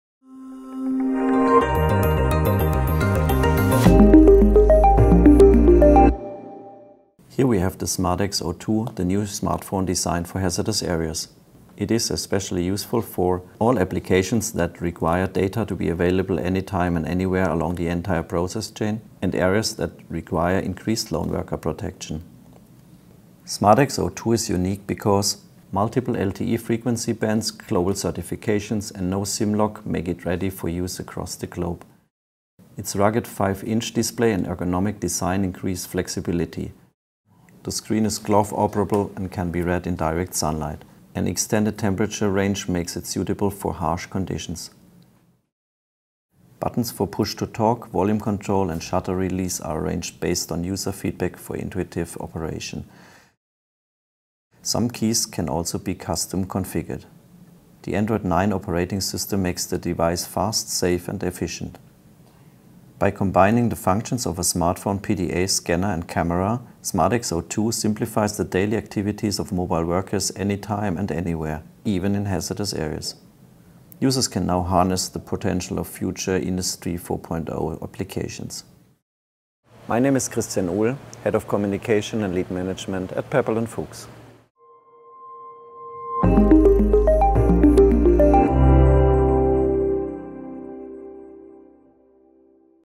Smart-Ex® 02—the State-of-the-Art Smartphone for Global Hazardous Areas
— The intrinsically safe Smart-Ex® 02 combines the functions of a smartphone, PDA, scanner, and camera to simplify the daily activities of mobile workers anytime and anywhere—even in hazardous areas. It has a large 5-inch display, an ergonomic design, advanced features, and optimized accessories. The 21 LTE capable, unlocked, and SIM-free Smart-Ex 02 is suitable for flexible use worldwide.